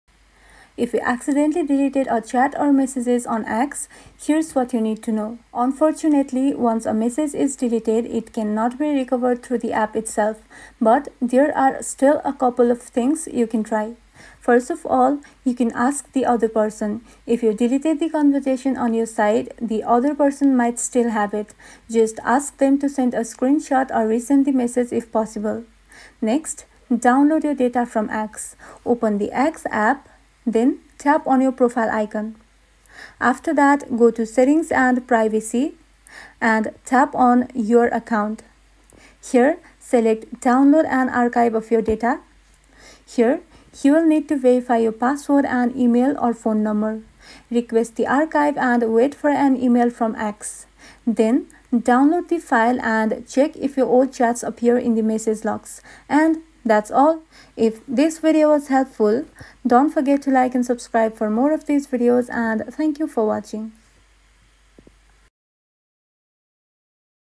How to Recover Deleted Chat On X formerly Twitter
00:00 – Intro
00:05 – Steps to Recover Deleted Messages on X (Twitter)

Open the X app and go to Settings → Account → Download Your Data. Request your data archive and wait for the email notification. Once received, open the archive to check deleted messages. For direct recovery, third-party backup apps or synced devices may help retrieve older chats. Always enable data backup to avoid permanent loss.